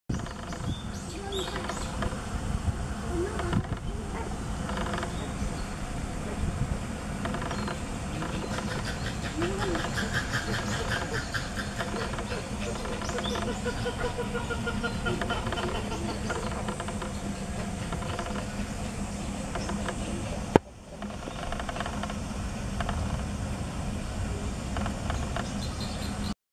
ツキノワグマの常同行動 のんほいパーク（豊橋総合動植物公園） 2020年7月24日
そこそこ広い場所にいますが、ほんの一部のところでぐるぐると周回しています。
こういうのを常同行動というのでしょうか。
動物園に入れられた動物ではよく見かける行動です。